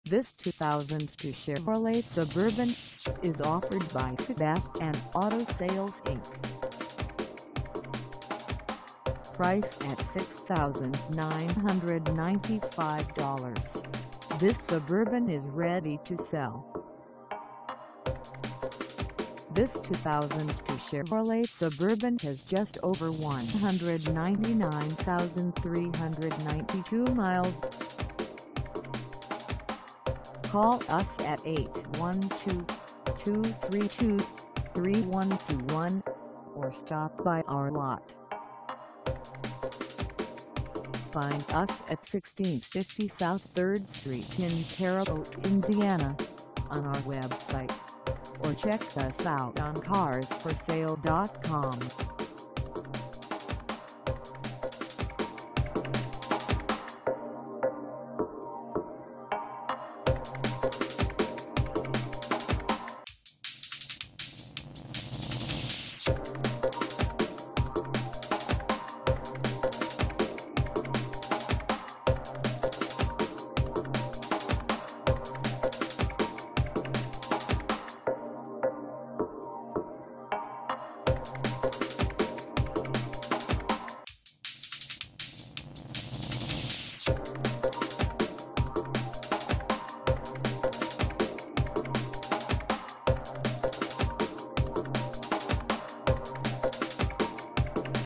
2002 Chevrolet Suburban 1500 LT 4WD 4dr SUV for sale in Terr
This 2002 Chevrolet Suburban 1500 LT 4WD 4dr SUV is for sale in Terre Haute, IN 47802 at SEBASTIAN AUTO SALES INC..
Engine: V8 5.3L V8
Transmission: Automatic 4-Speed
Color: GREEN
Mileage: 199392
Contact SEBASTIAN AUTO SALES INC. 1650 S 3RD ST Terre Haute, IN 47802 to test drive this 2002 Chevrolet Suburban 1500 LT 4WD 4dr SUV today.